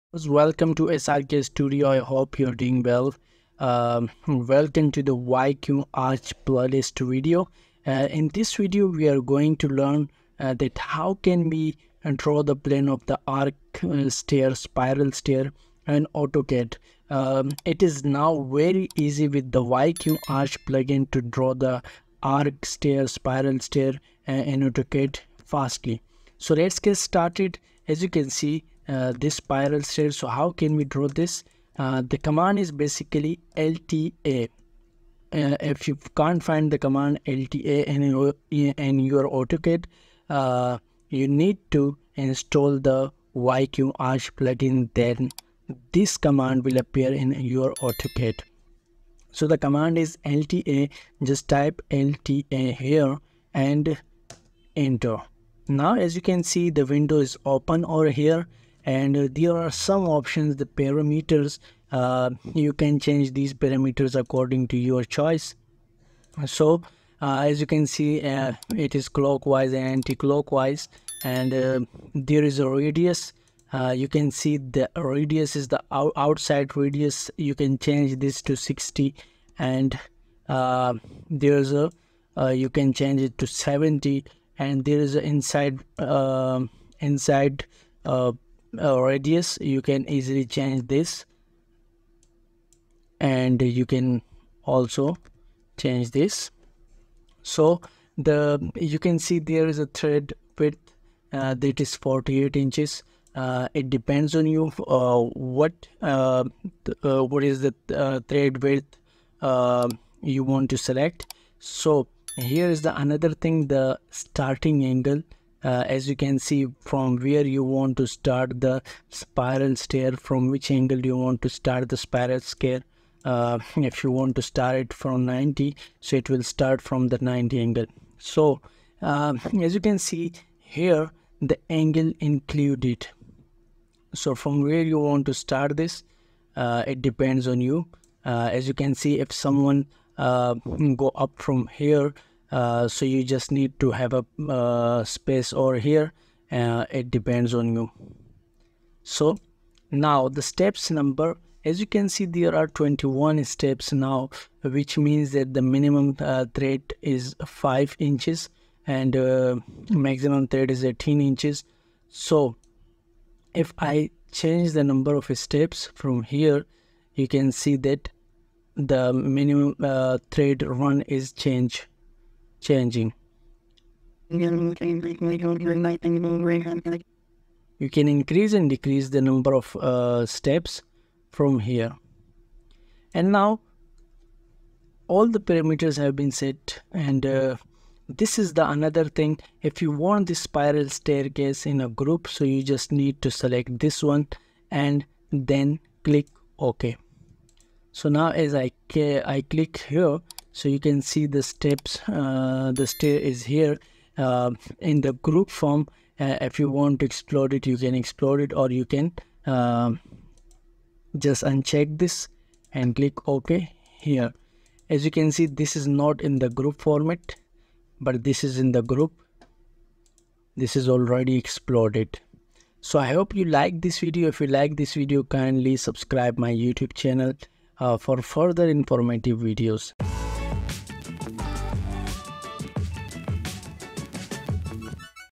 Mind-Blowing Spiral Stair Design in Autocad: A Step-by-Step Guide
In thi tutorial we are going to learn that how quickly we can Draw plan of spiral staircase in autocad with the help of Yqarch plugin.